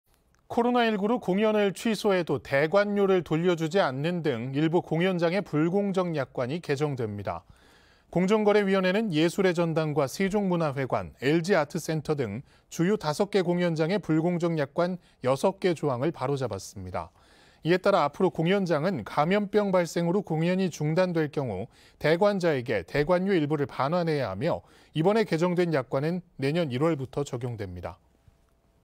[KTV] "코로나로 취소 땐 대관료 환불"··· '공연장 갑질' 사라진다
"코로나로 취소 땐 대관료 환불"··· '공연장 갑질' 사라진다